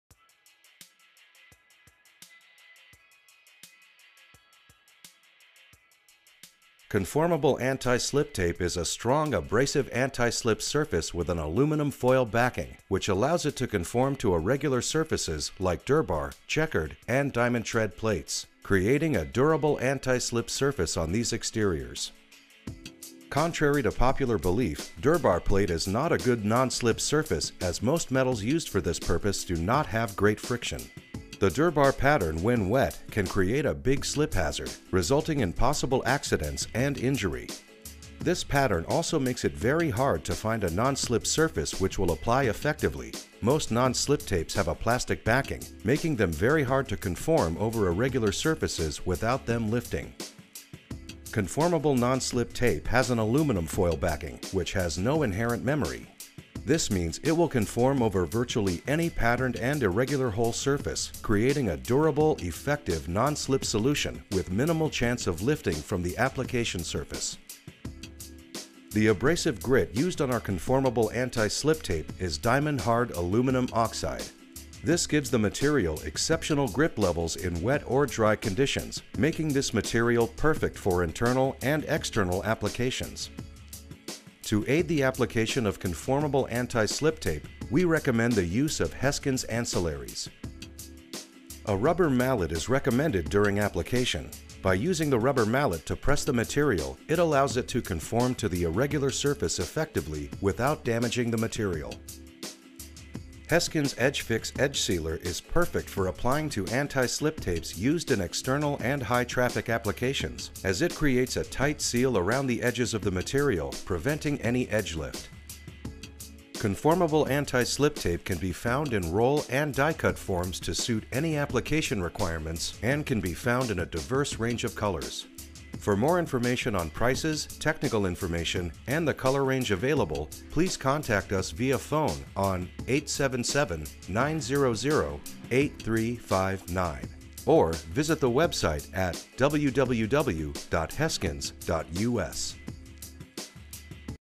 Conformable Anti Slip Tape
Conformable anti slip tape is a strong abrasive anti slip surface with an aluminum foil backing, which allows it to conform to irregular surfaces like durbar, chequered & diamond tread plates, creating a durable anti slip surface on these exteriors.
Contrary to popular belief, Durbar plate is not a good non-slip surface as most metals used for this purpose do not having great friction. The durbar pattern, when wet, can create a big slip hazard, resulting in possible accidents and injury. This pattern also makes it very hard to find a non slip surface which will apply effectively; most non slip tapes have a plastic backing, making them very hard to conform over irregular surfaces without them lifting.
Conformable non slip tape has an aluminium foil backing, which has no inherent memory. This means it will conform over virtually any patterned and irregular whole surface, creating a durable, effective non-slip solution with minimal chance of lifting from the application surface.
The abrasive grit used on our conformable anti slip tape is diamond hard aluminum oxide. This gives the material exceptional grip levels in wet or dry conditions, making this material perfect for internal and external applications.
To aid the application of conformable anti slip tape, we recommend the use of Heskins ancillaries.
A rubber mallet is recommended during application. By using the rubber mallet to press the material it allows it to conform to the irregular surface effectively without damaging the material.
Heskins Edge fix edge sealer is perfect for applying to anti slip tapes used in external and high traffic applications as it creates a tight seal around the edges of the material, preventing any edge lift.
Conformable anti slip tape can be found in roll and die cut forms to suit any application requirements and can be found in a diverse range of colors.